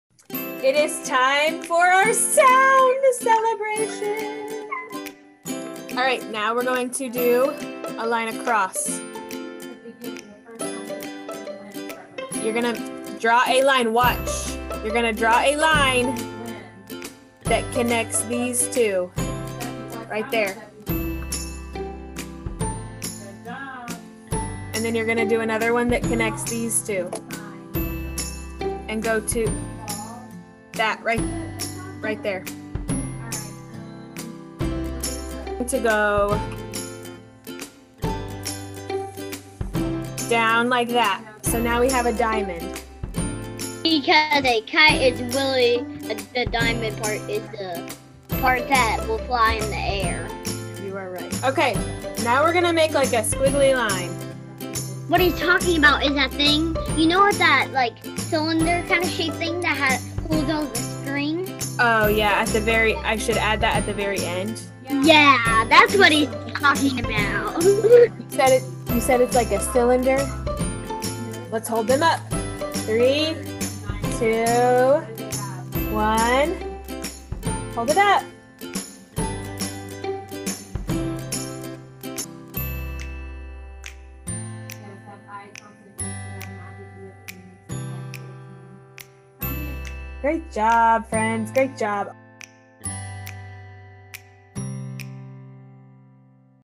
ISD Virtual First Grade Teacher Celebrates Student Achievement
Each week, students in Ms. Penniston's virtual first grade class work on learning a new sound and letter combination. At the end of the week, they hold a sound celebration with a guided drawing lesson related to the sound they mastered! Watch as students draw kites together after a successful week here...